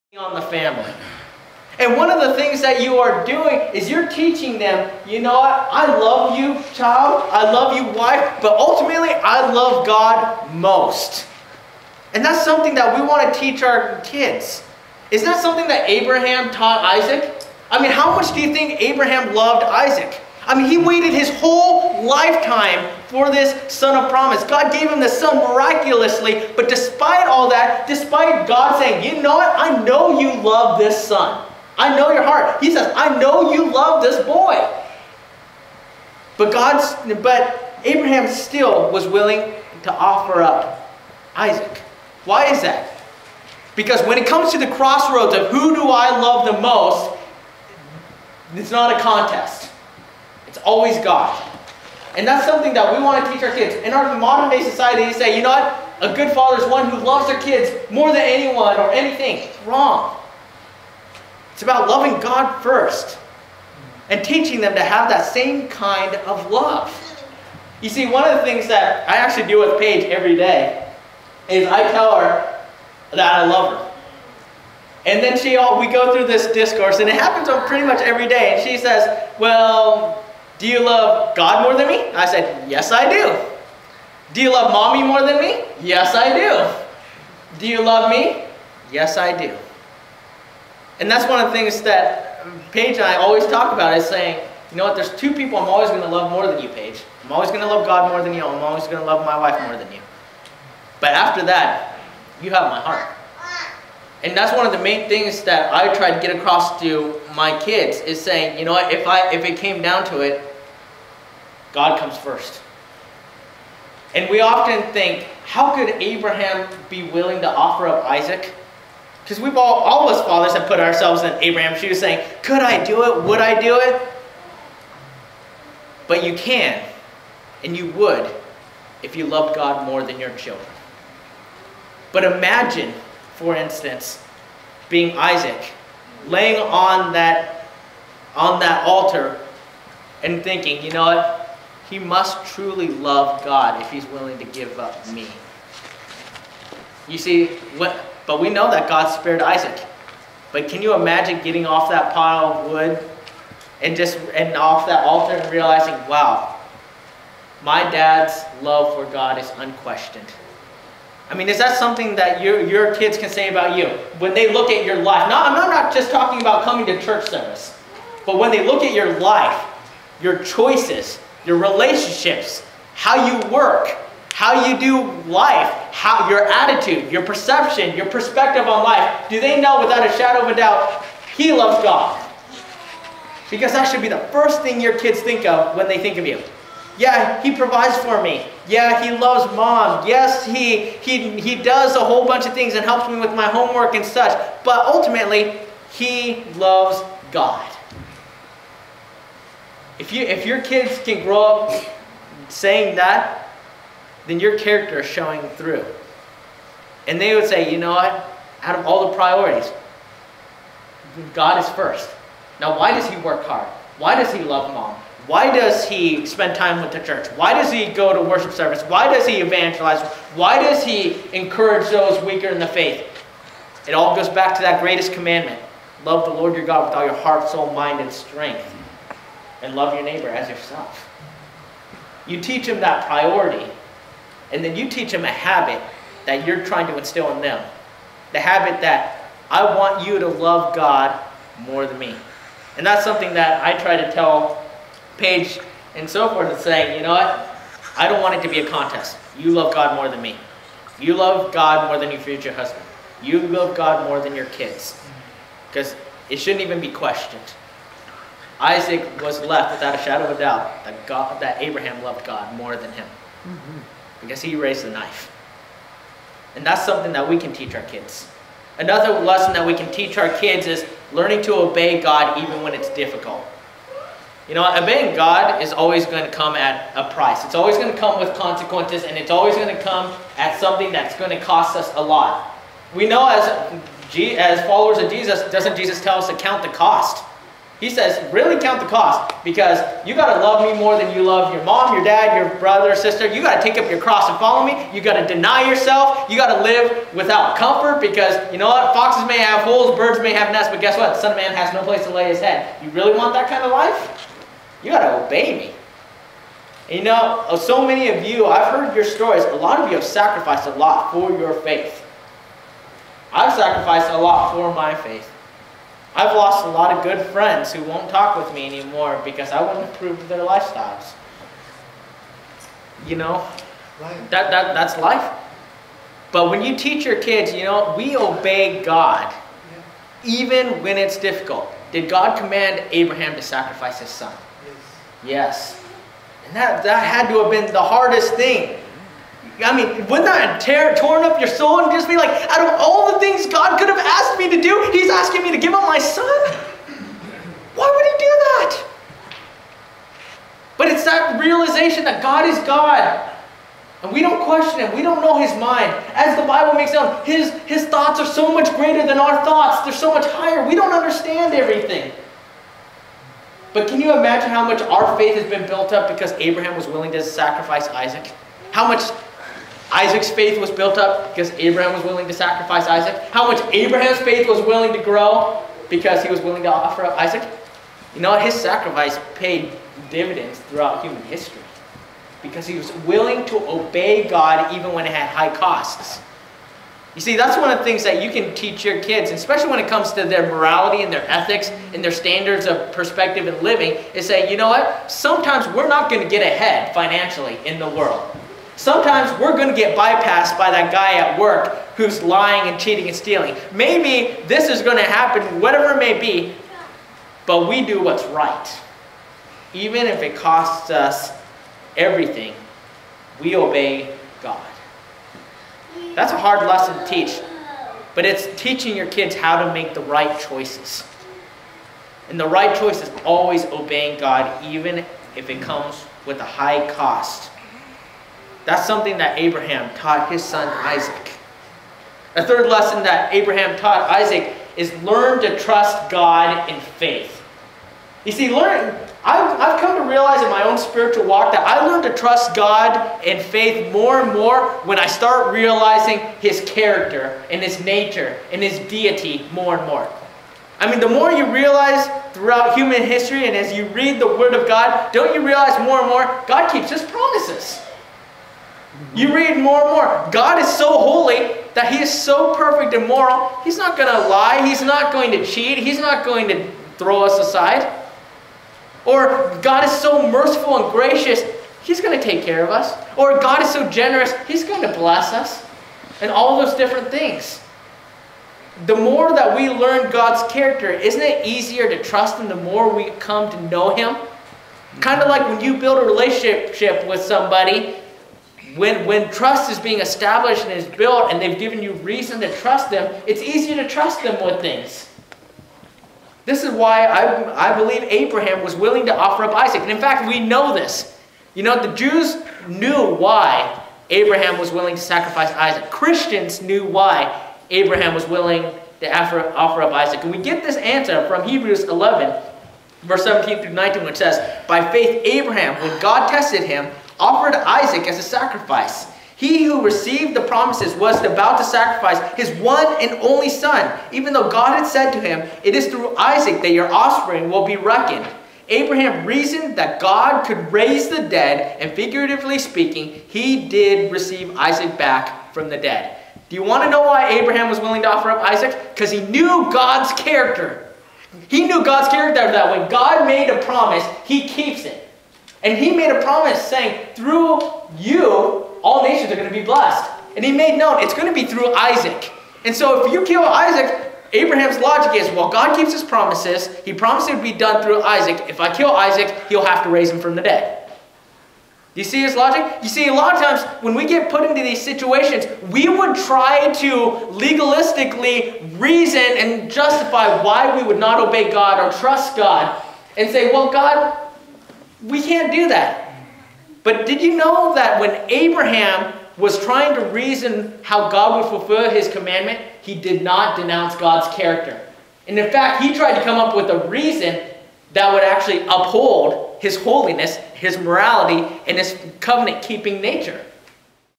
Courage to Be a Father - Part 2
Sermon Preached by Micah Powless, June 15, 2014, Moses Lake church of Christ